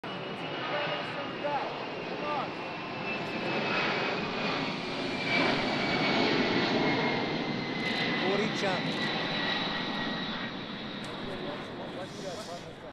PM Netanyahu watches the US 'Supertanker' fire-fighting aircraft
PM Netanyahu watches the US 'Supertanker' fire-fighting aircraft as it takes part in efforts to extinguish the fire in Israel's North. 5.12.10
Stay updated with the PM's social media sites